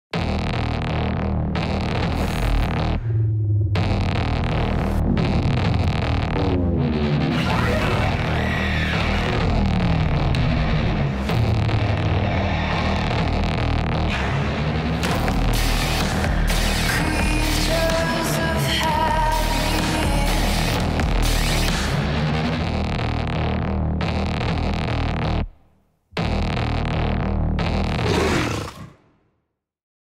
2017 Jaguar F-PACE | Jaguars roar. | Jaguar Canada
Birds fly. Fish swim. Jaguars roar.

The Jaguar F-PACE is officially the best and most beautiful car in the world, having been crowned 2017 World Car of Year and World Car Design of the Year.